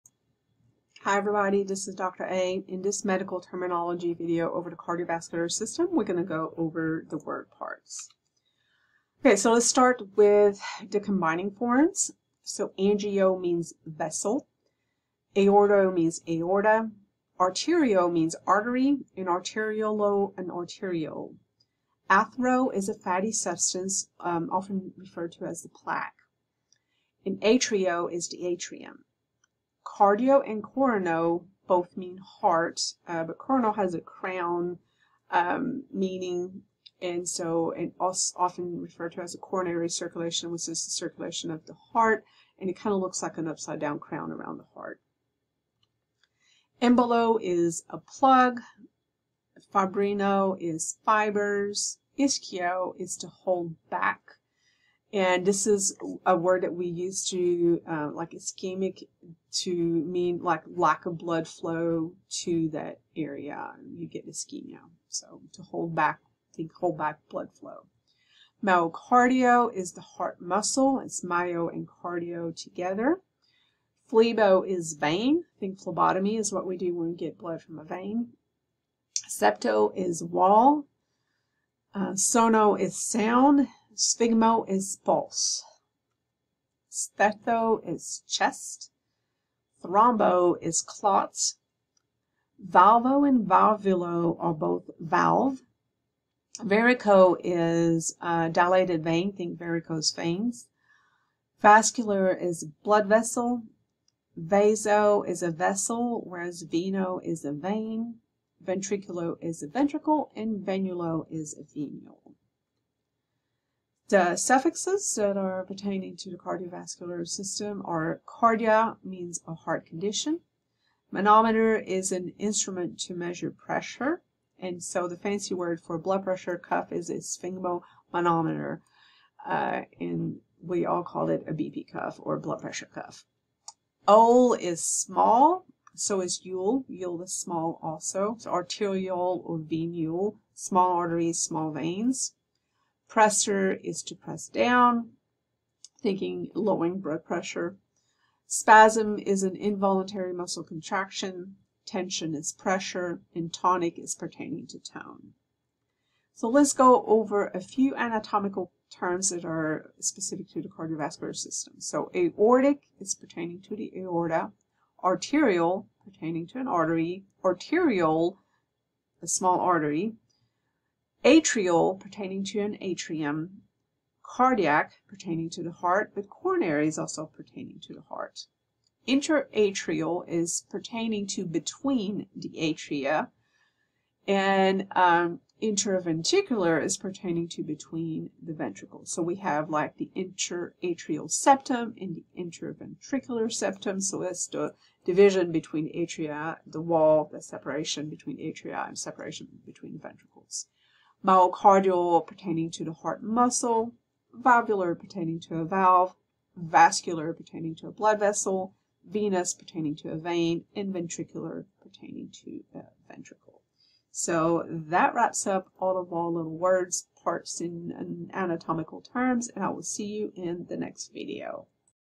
Medical Terminology of the Cardiovascular System Word Parts
The combining forms, suffixes, and anatomical terms of the cardiovascular system

Based on Medical Terminology: A Living Language by Fremgen and Frucht. Published by Pearson.